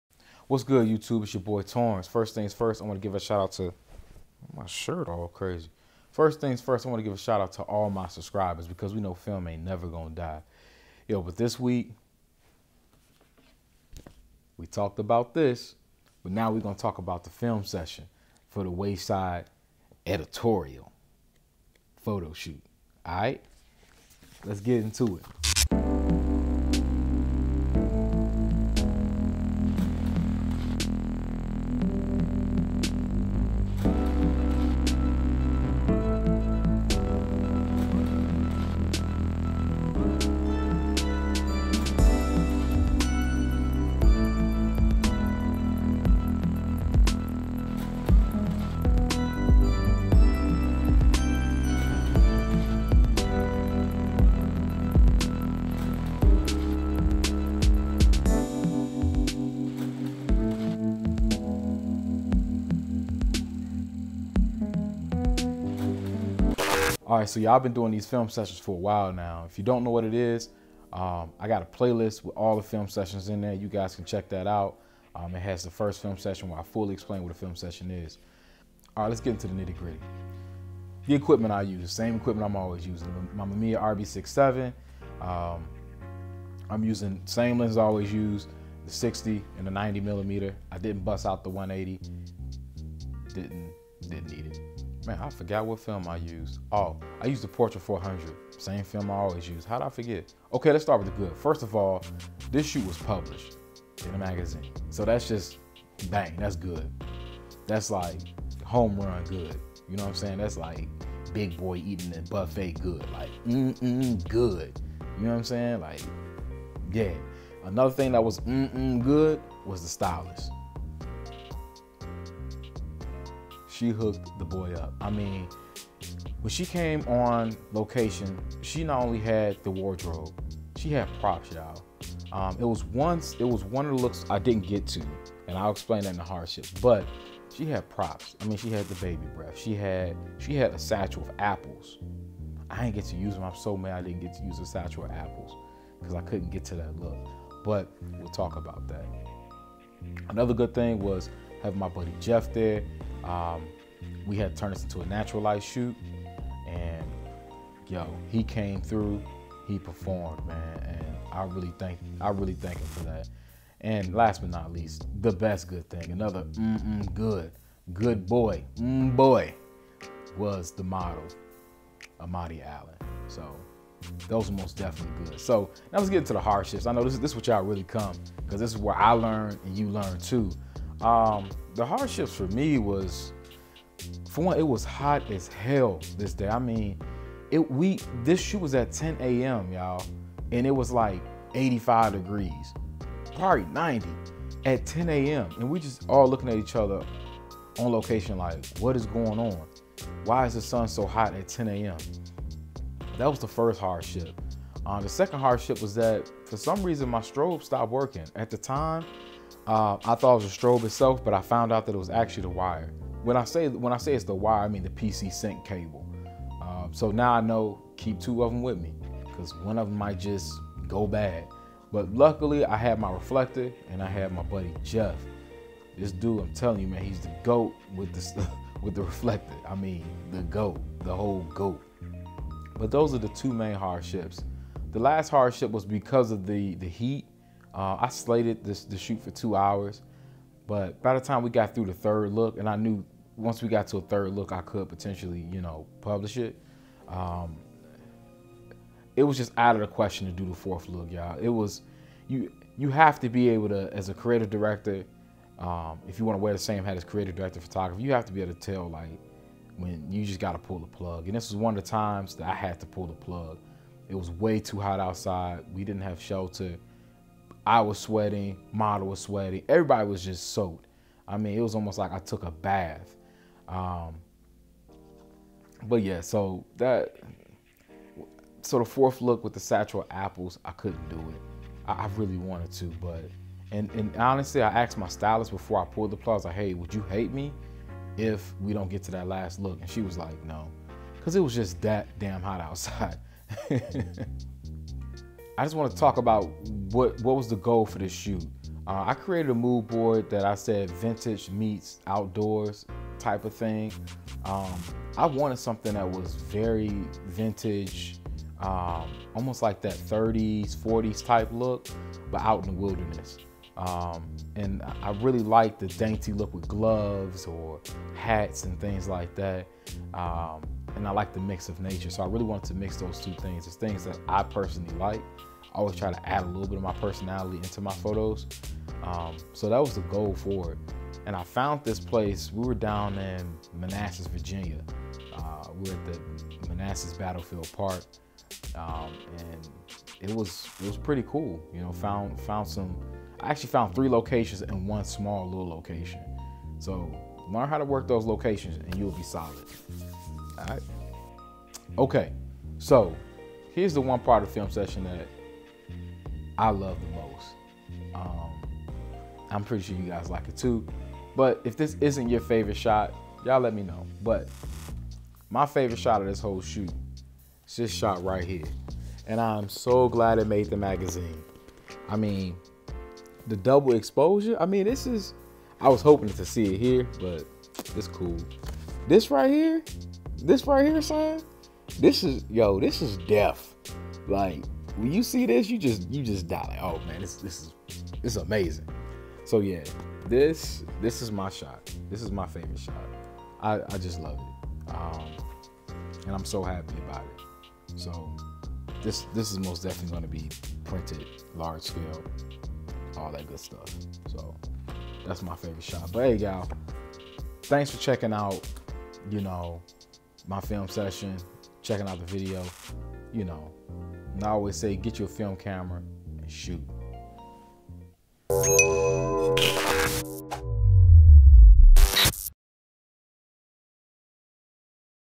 Film Sessions: Mamiya RB67 Film Photography Fashion Photo shoot. Published Edition!
Y'all know what time it is! Film Session time! Learn what I like, love and hated about my fashion film photography shoot.

Film Session Ep. 1